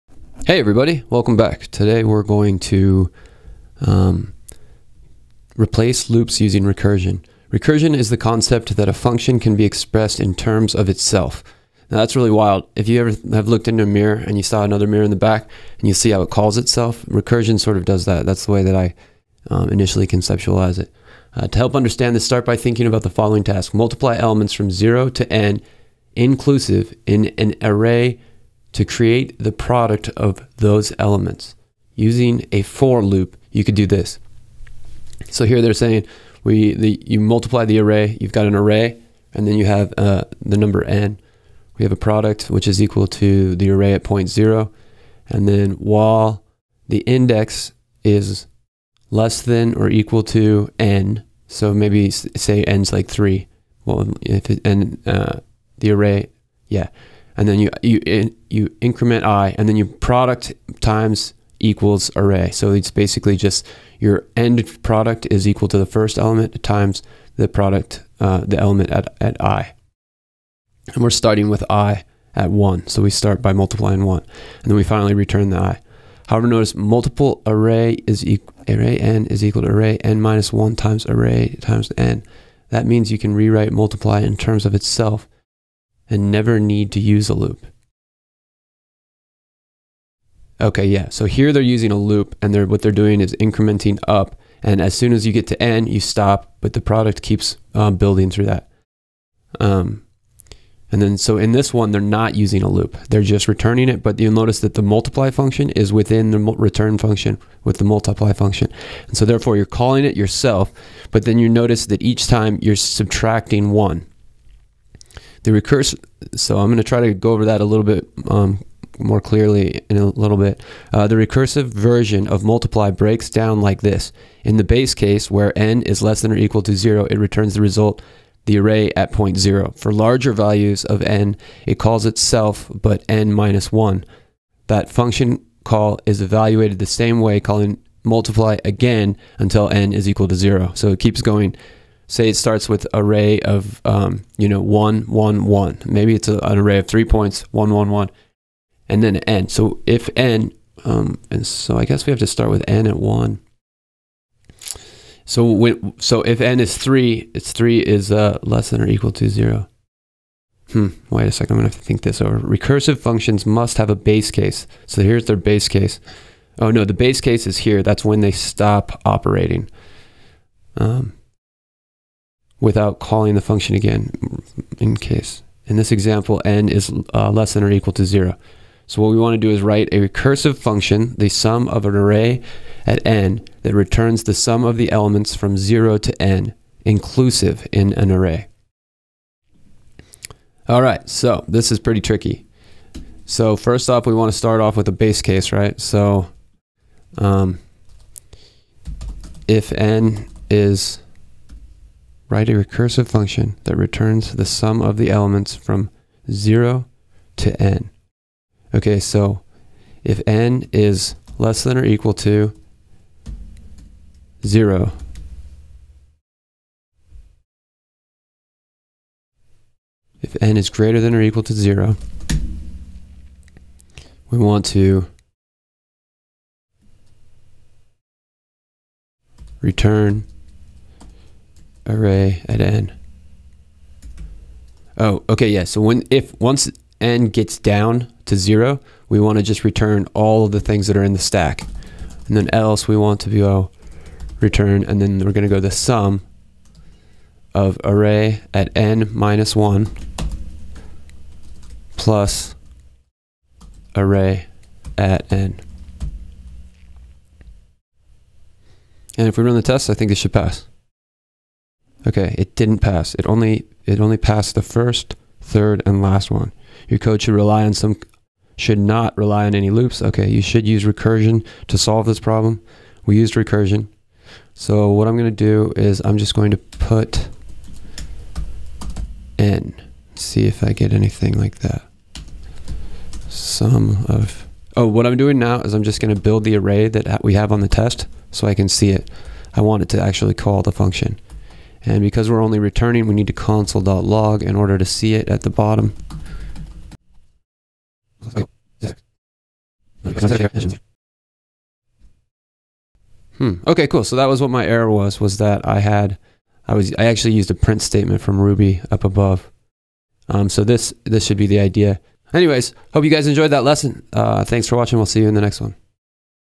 Replace Loops using Recursion - Free Code Camp
This is a basic JavaScript tutorial where we replace loops using recursion. Recursion is quite difficult for me to get my head around in general.